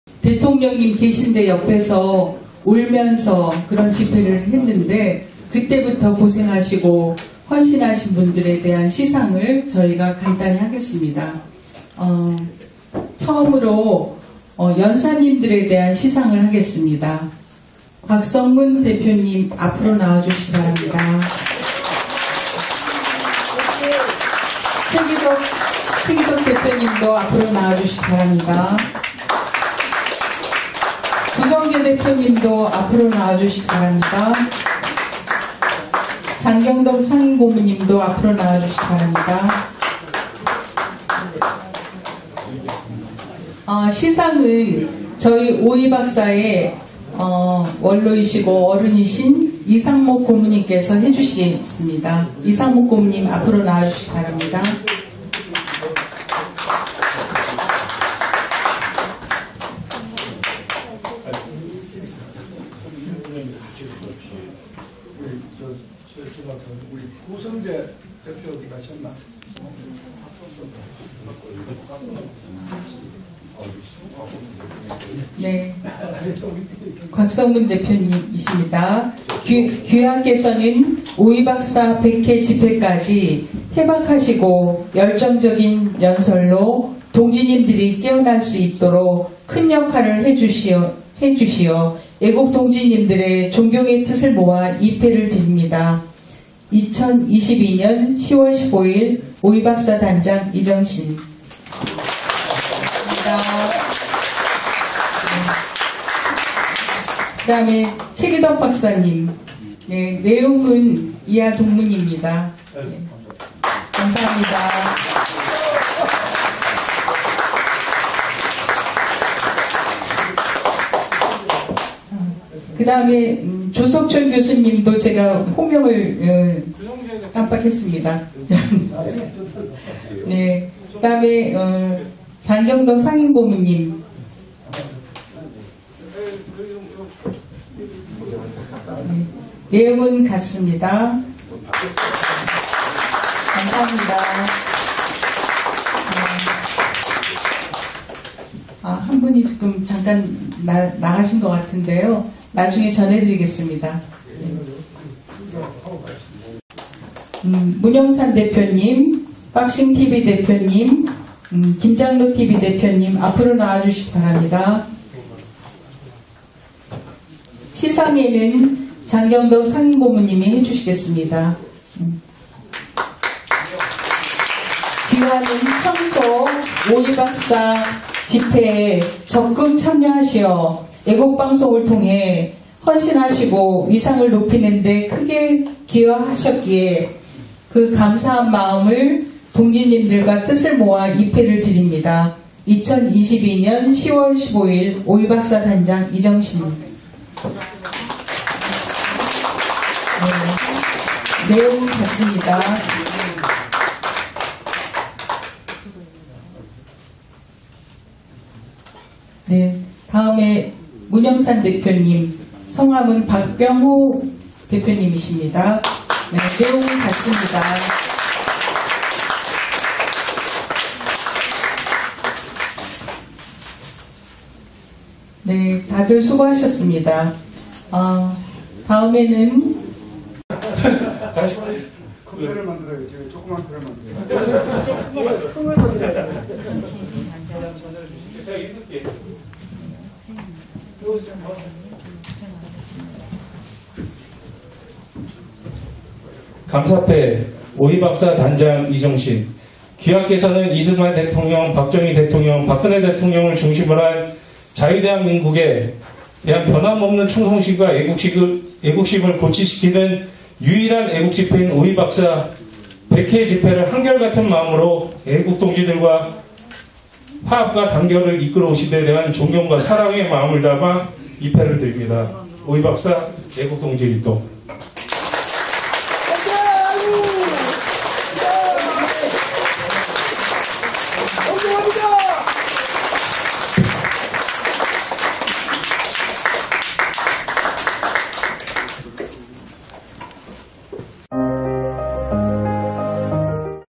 [단체 사진 및 공로자 시상식] 오이박사 집회 100주년 기념 세미나 / 서울시의회 별관 4층 2022.10.15 -지역에서 세계로, 안동데일리TV, Since 2016
[자율구독료 납부안내]